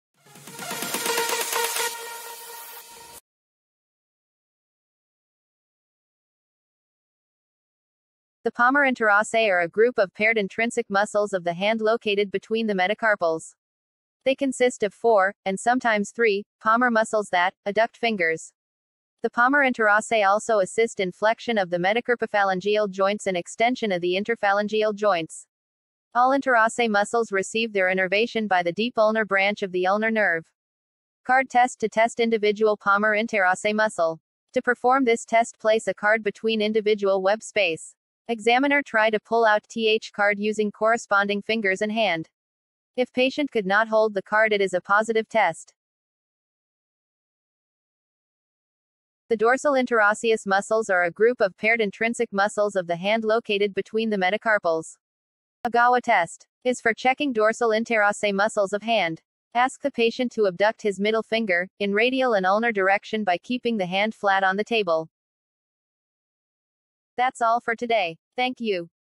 Card test,Egawa test,ulnar nerve palsy,claw hand
Card test and Egawa test are two special tests for Ulnar nerve.Ulnar nerve supply palmar and dorsal interrossei.Weakness of these muscles results in positive test.
ulnar nerve
Examination of nerves